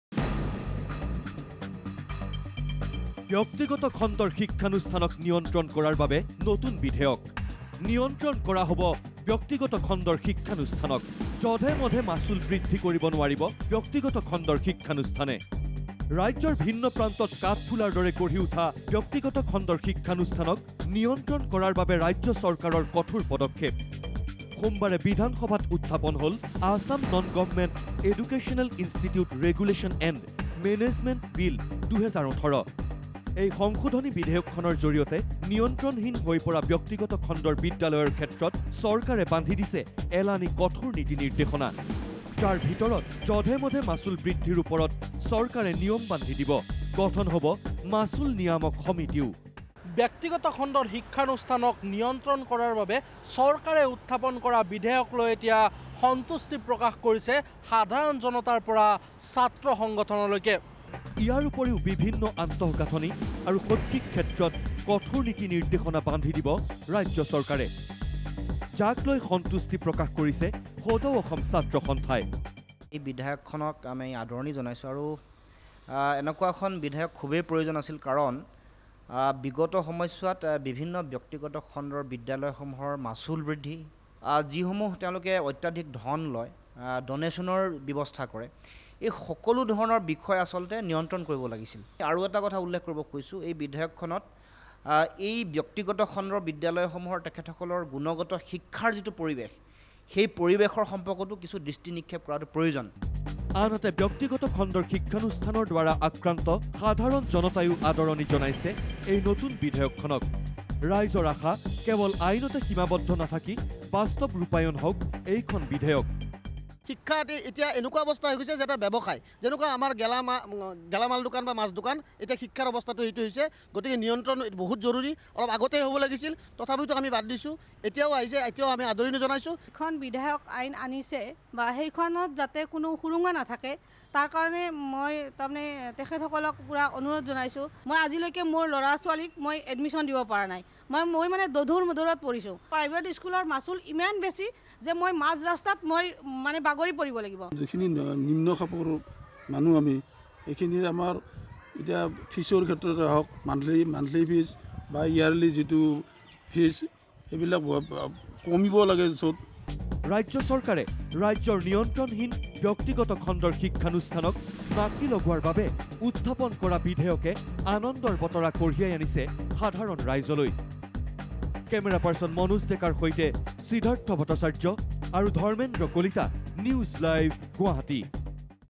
Govt. reins private schools from increasing fees, guardians happy
ব্যক্তিগত খণ্ডৰ বিদ্যালয়ক নিয়ন্ত্ৰণৰ বাবে বিধানসভাত উত্থাপন নতুন বিধেয়ক। জধে-মধে মাচুল বৃদ্ধি কৰিব নোৱাৰিব ব্যক্তিগত খণ্ডৰ শিক্ষানুষ্ঠানে। নতুন বিধেয়কক লৈ সন্তুষ্টি সাধাৰণ ৰাইজৰ।